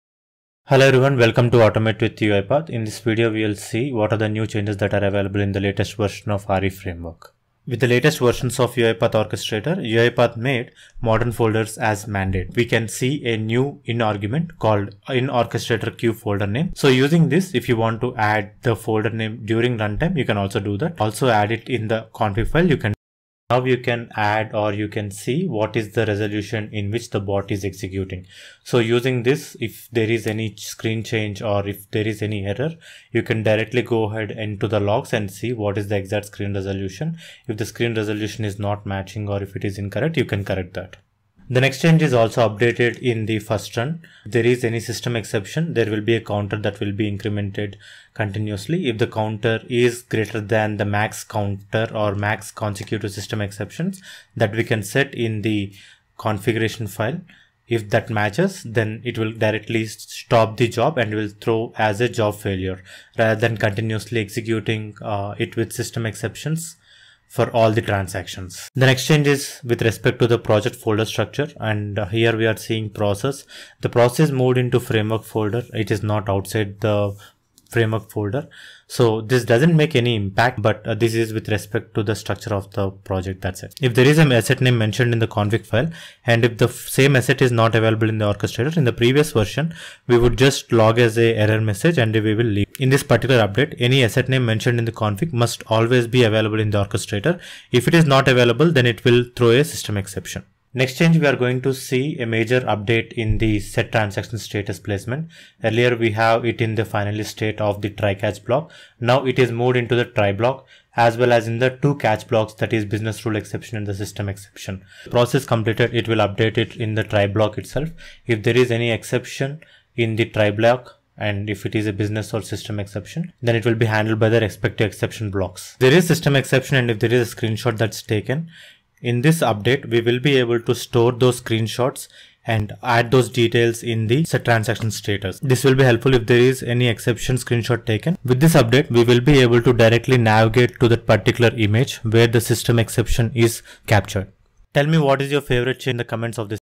UiPath ReFramework Changes in 2021.10 release UiPath
Live RPA projects for Dec 1 2021
vajrangtalks ReFramework Changes in 2021.10 release
uipath Reframework improvements that were released in 21.10 latest version

* Added Orchestrator Queue folder name in config and also an input argument in Main

* Process.xaml moved to the framework folder

* SetTransactionStatus.xaml
- was added after process.xaml in the same try block
- was also added for each type of exception (System or Business)
*** That means it is no longer in the finally block***

* Added maximum consecutive retry number for persistent system exceptions. If that retry number was reached then the job is marked as failed

* Failed queue items updates with the location of associated exception screenshot

* In the init state the screen resolution can be logged

* If the declared asset in config (Assets sheet) is missing in orchestrator then the process stops in the init phase itself, which was not happened in the previous versions

* Also added some test cases related to the workflows that uses new features of Test Suite